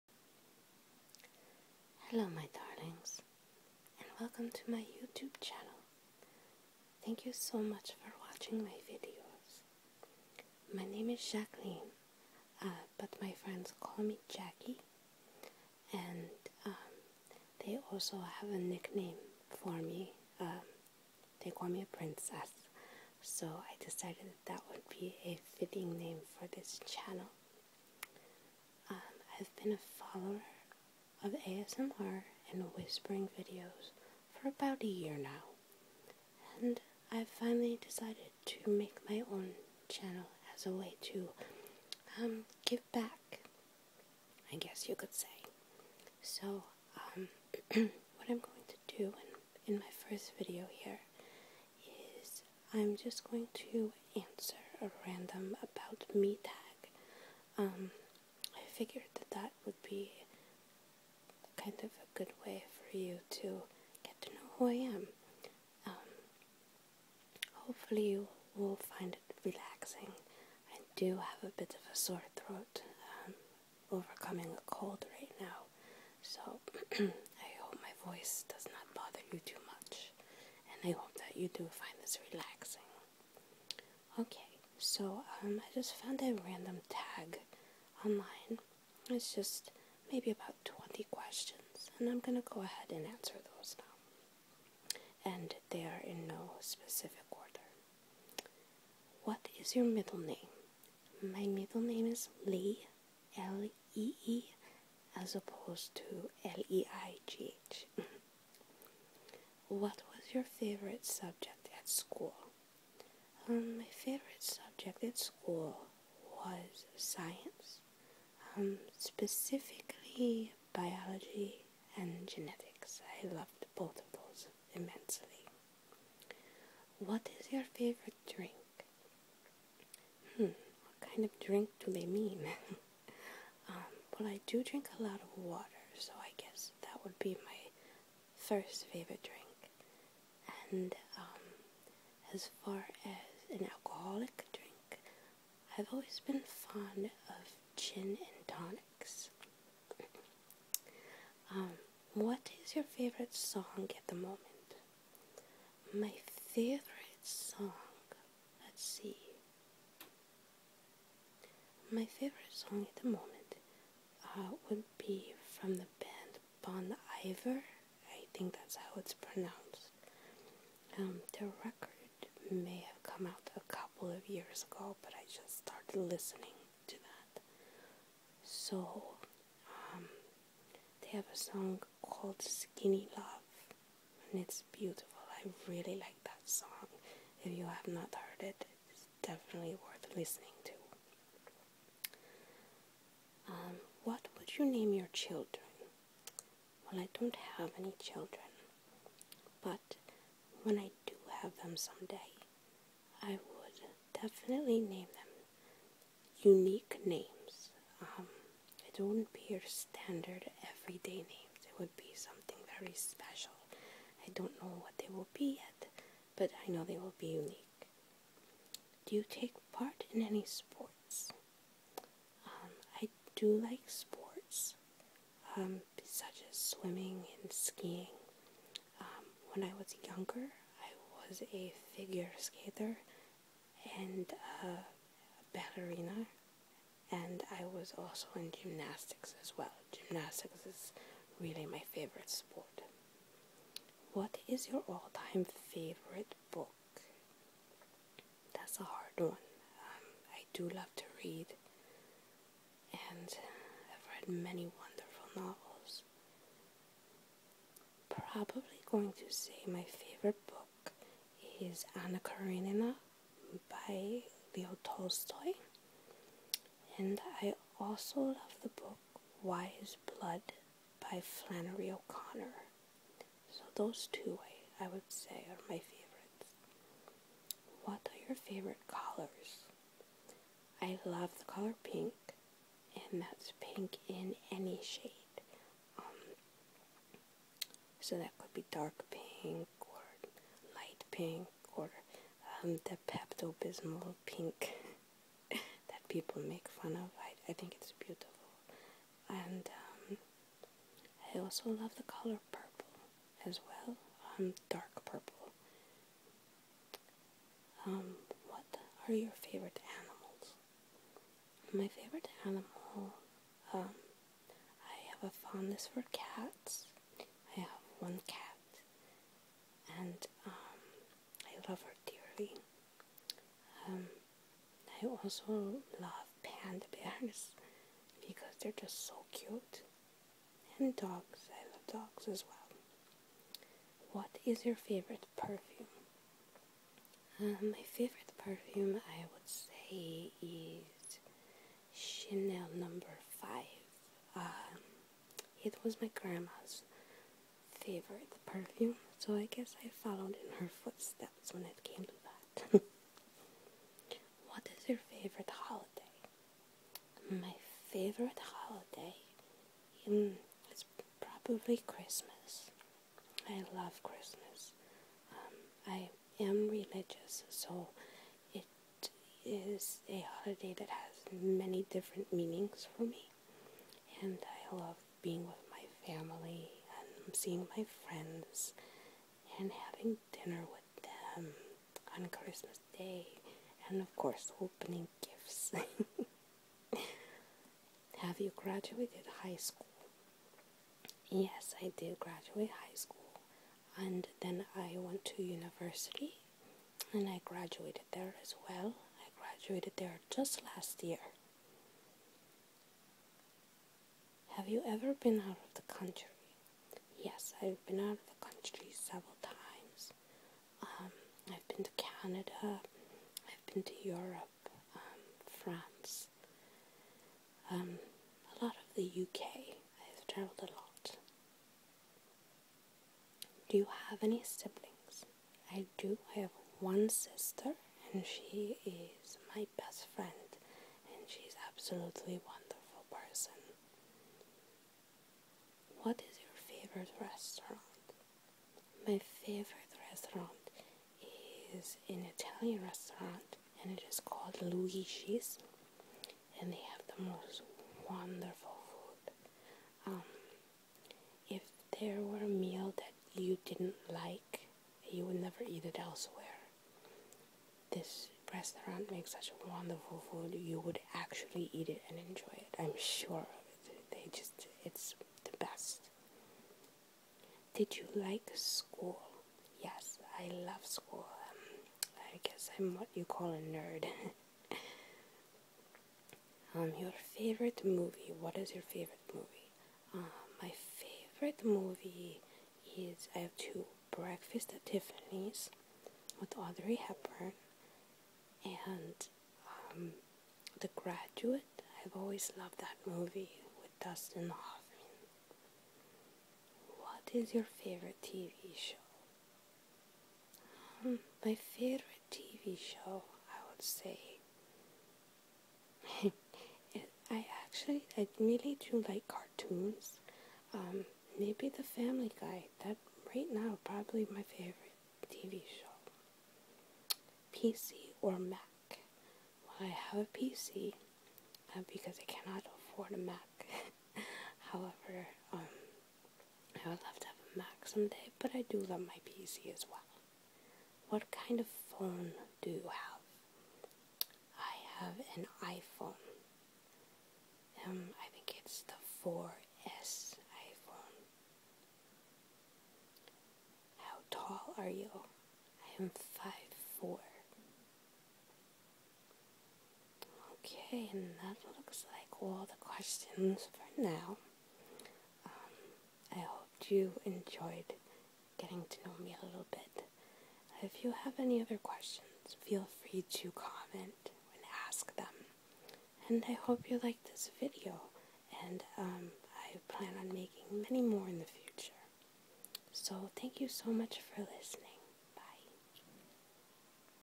"Getting to Know You" Tag
Hello darlings! Here is a "getting to know you tag" about me.. I would say it is a semi whisper due to my sore throat! I hope you find it relaxing!